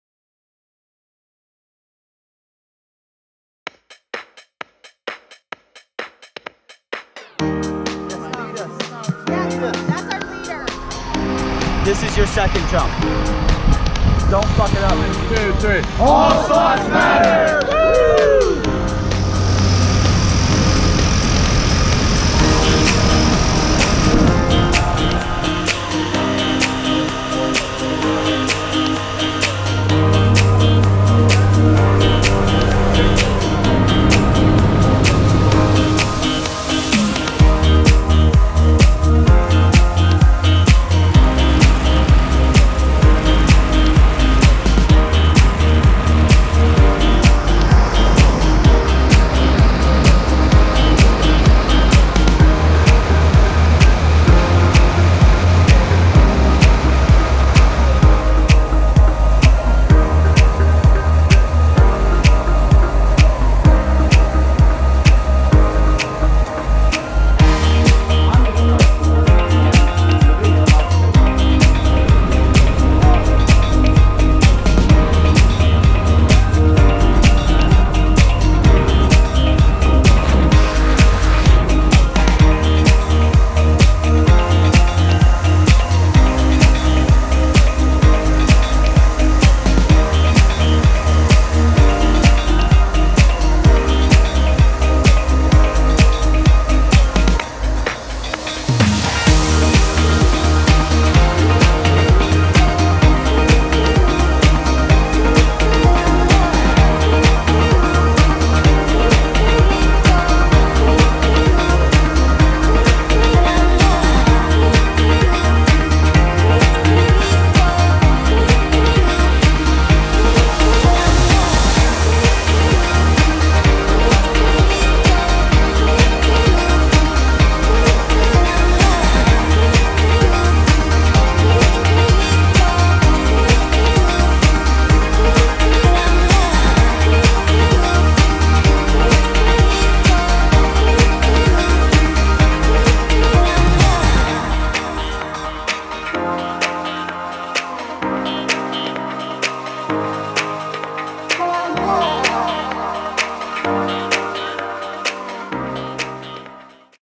Illinois Wingsuit State Record
At summerfest 2017, we made 14 jumps to breake the state record.
A new record was set : 29 wingsuit in a grid formation !
Big Thanks to the organisers and Skydive Chicago !